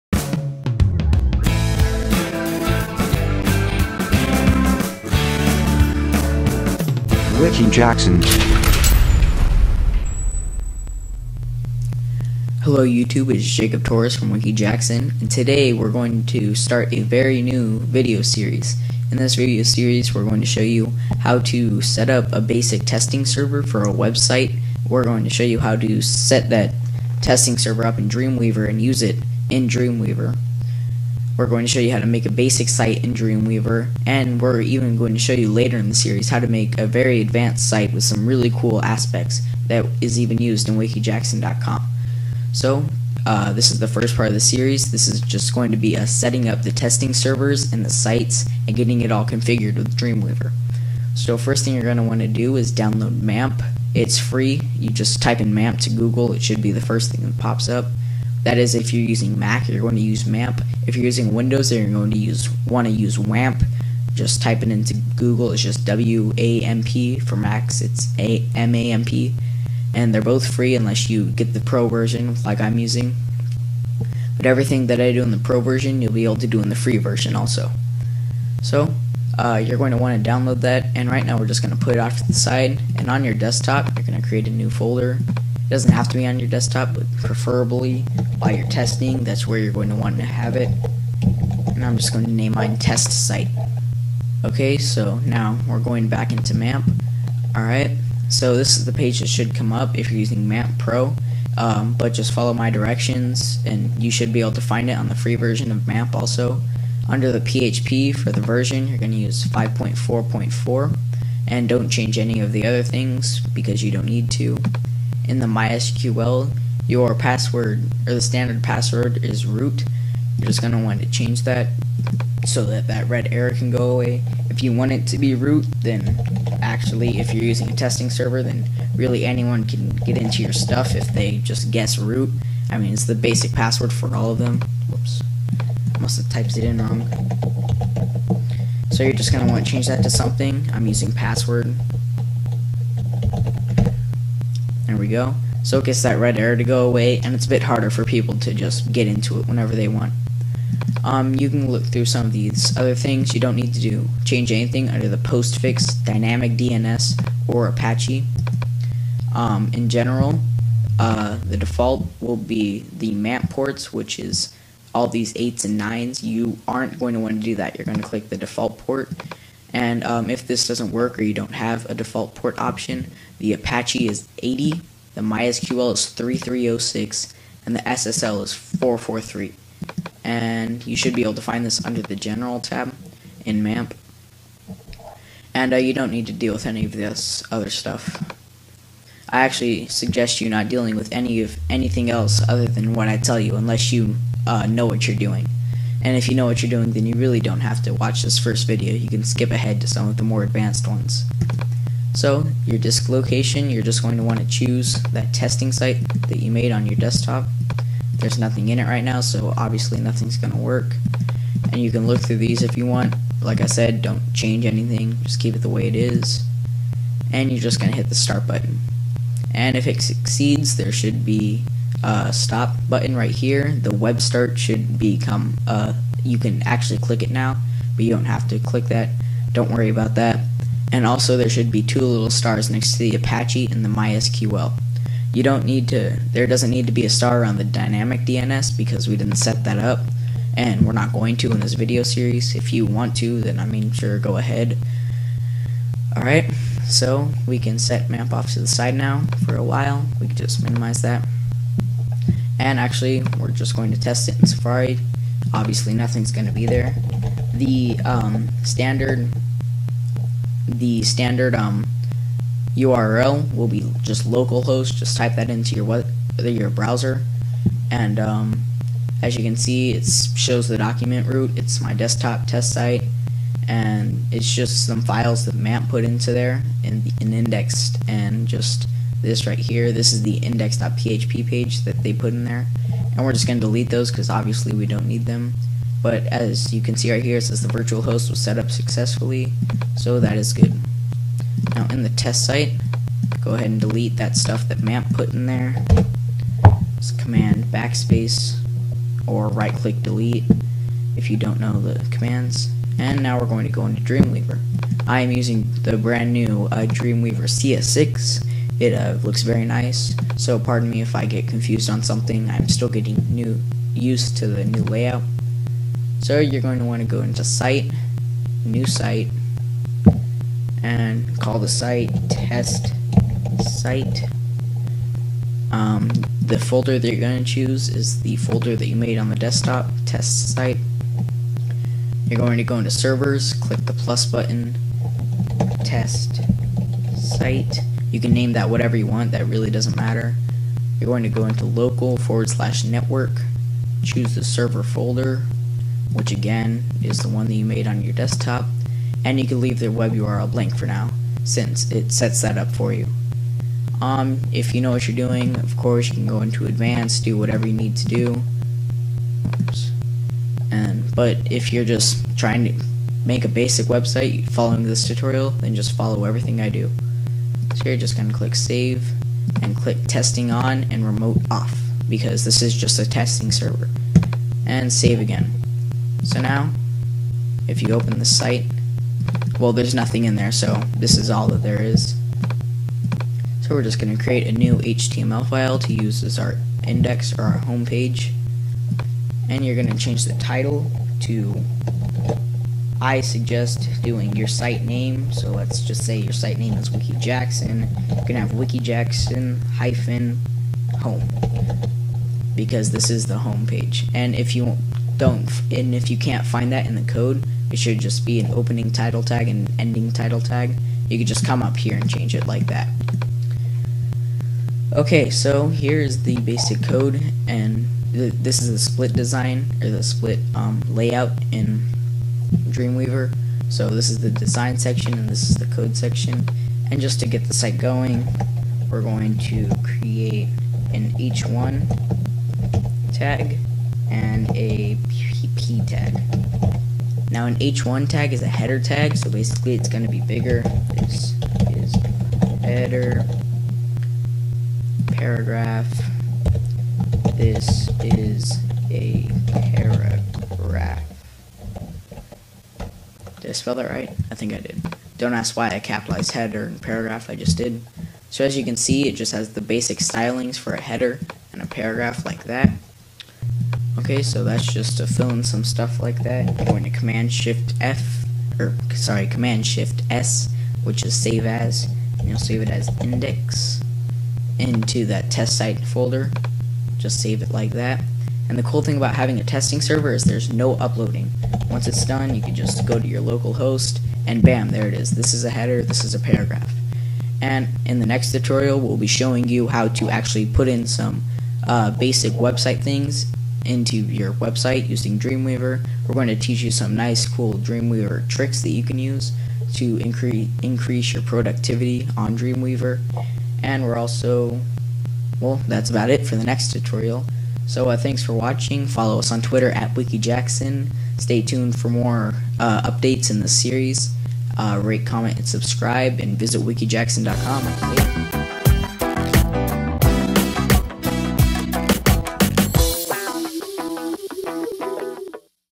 Dreamweaver Tutorial #1- Setting up a local testing server
In the first installment of this series, i will be showing you how to set up a local testing server using MAMP, and i will show you how to setup the sever and site in Dreamweaver.